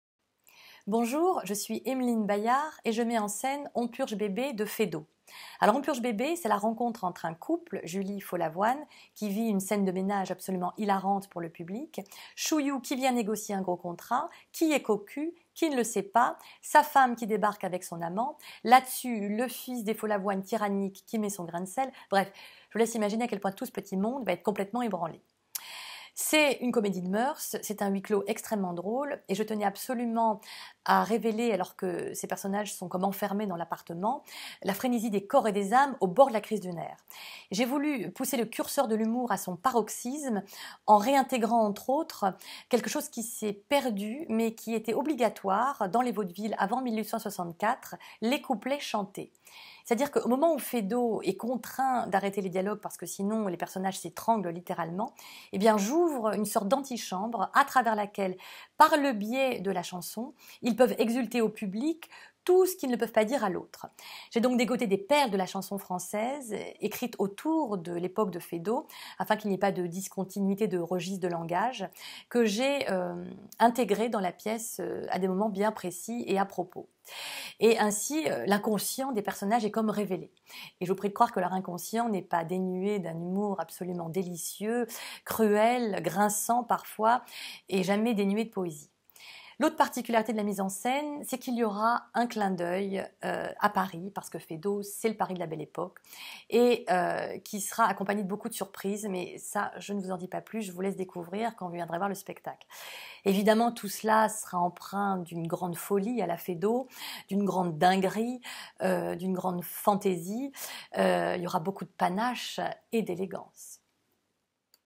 On purge bébé orchestré par Emeline Bayart
A partir du 13 octobre 2020 au théâtre de l'atelier, Emeline Bayart va mettre en scène On purge bébé de Feydeau. Elle nous en dit plus sur cette comédie de moeurs!